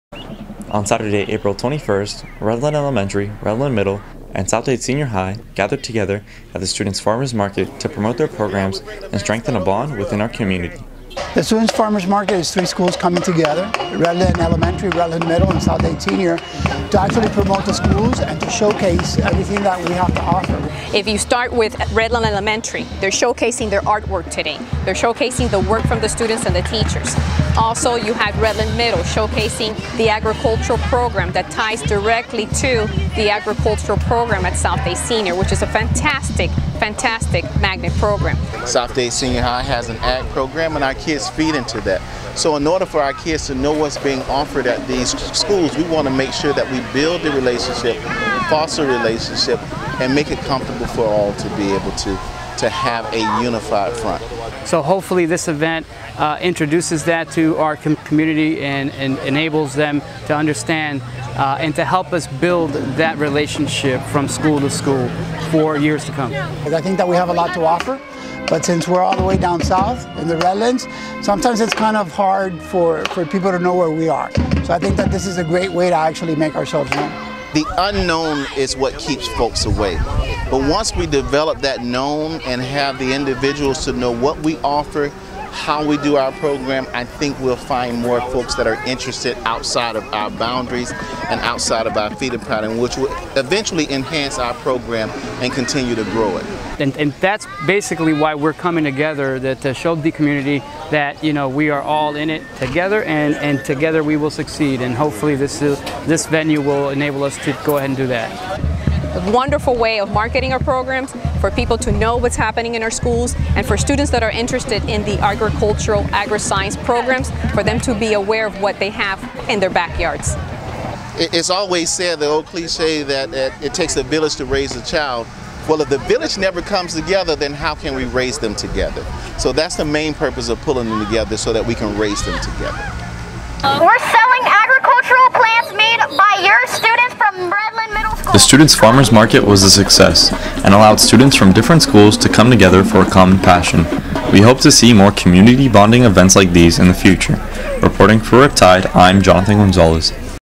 2018 Student Farmer's Market
On Saturday, April 21, 2018, South Dade High School, Redland Middle School and Redland Elementary School came together for the first annual Student Farmer's Market.